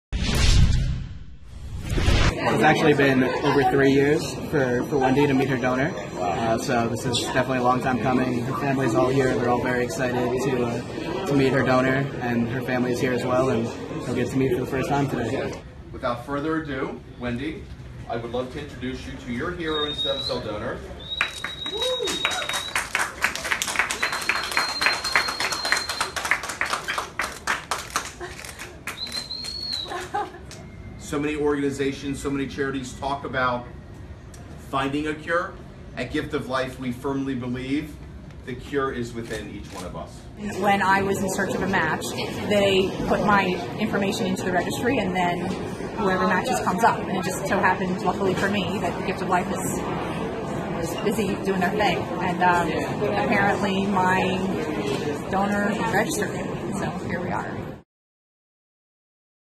Mets help Gift of Life set up special meeting - MLB com
Wendy Wax meets her blood stem cell donor Ruchail Feldman for the first time at Citi Field in May 2017. Gift of Life Chairman Bill Begal introduced them.